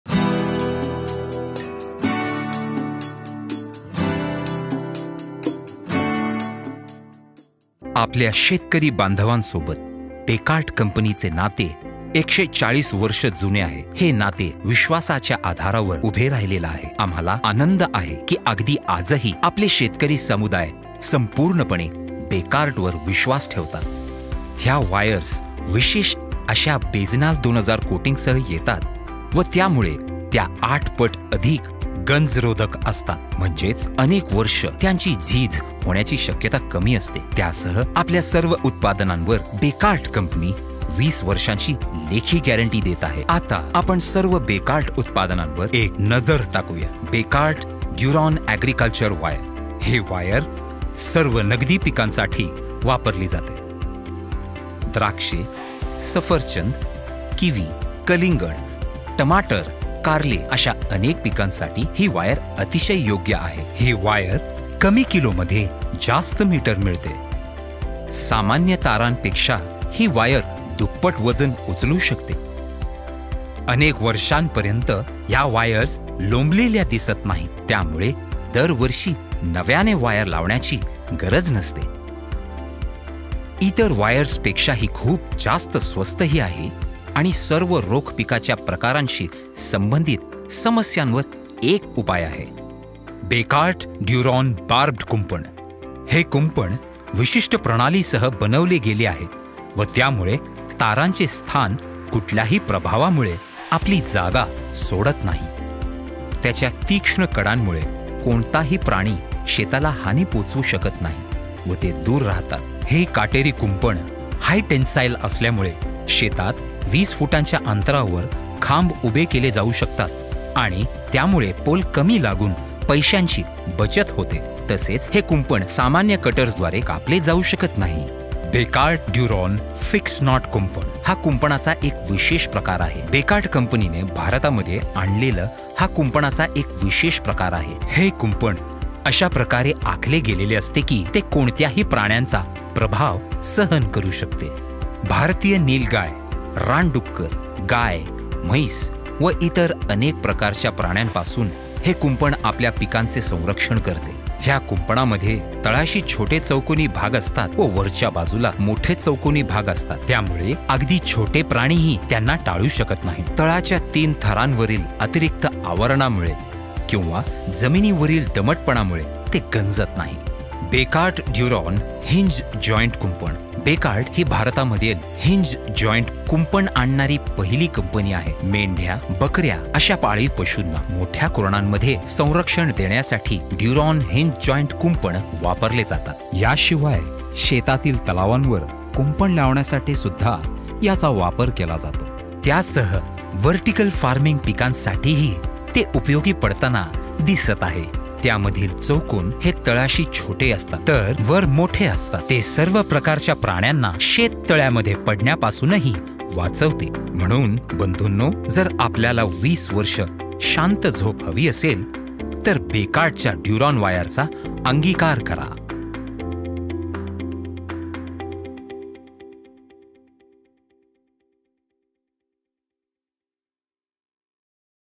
Bekaert Duron Product Portfolio | Marathi
Bekaert Duron products represent the complete dedication towards the cause of protecting and supporting Indian farming practices. Our product range promises to be durable and strong unlike any conventional products available today. With expert engineering and a precise aim to provide one-of-a-kind service, Bekaert is now taking shape as the most trusted fencing product within farming communities.

To know about our premium product range, Get in touch with us on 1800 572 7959 or visit us @